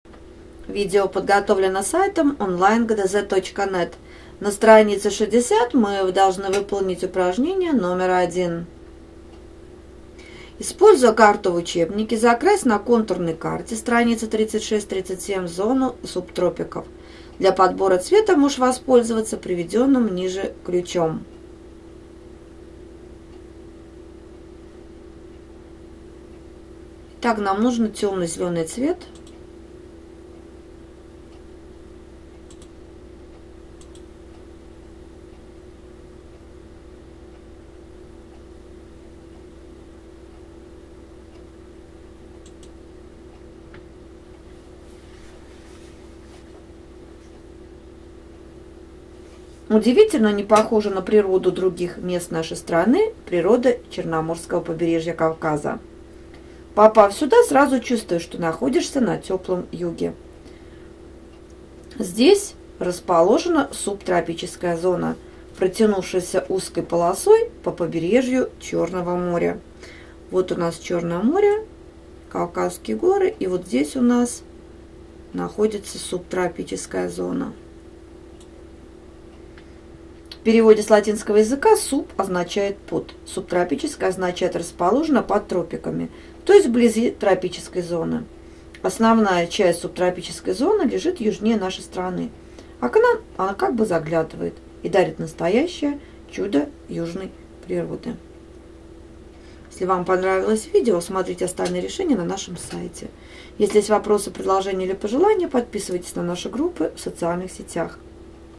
Задание 1 У Чёрного моря - Окружающий мир 4 класс (Плешаков А.А.) 1 часть
Для того чтобы выполнить задание №1 на странице 60, вы должны уметь на контурных картах закрашивать зону субтропиков. А подсказкой для вас будет наше видео с решением!